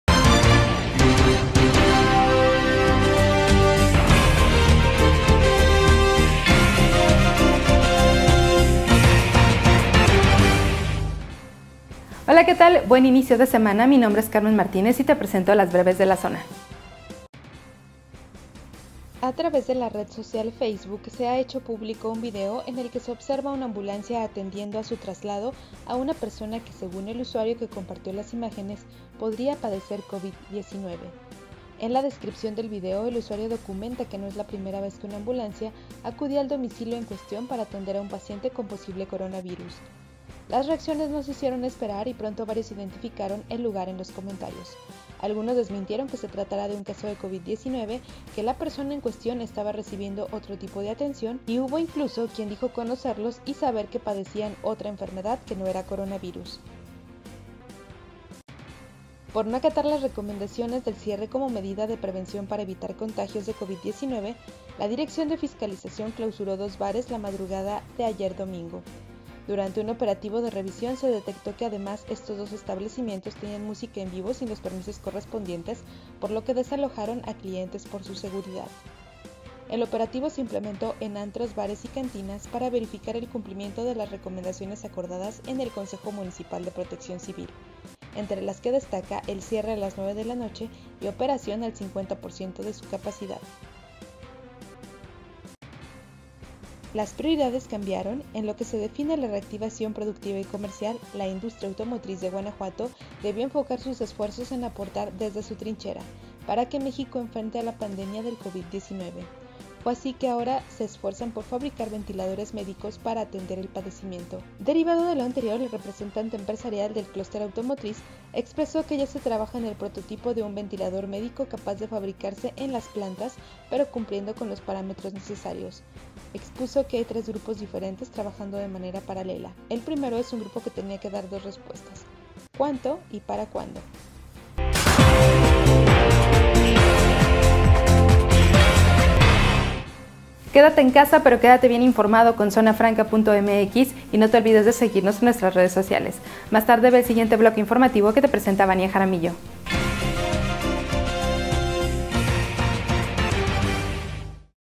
Es Guanajuato la segunda entidad con más tomas clandestinas | Breves de la Zona
BrevesDeLaZona📰 | Esto es lo más destacado de este lunes
- Vecinos graban atención de ambulancia a posibles contagiados de COVID19 en León.
- En GuanajuatoCapital clausuran dos bares por no acatar recomendaciones ante el COVID19.
- Es Guanajuato la segunda entidad con más tomas clandestinas.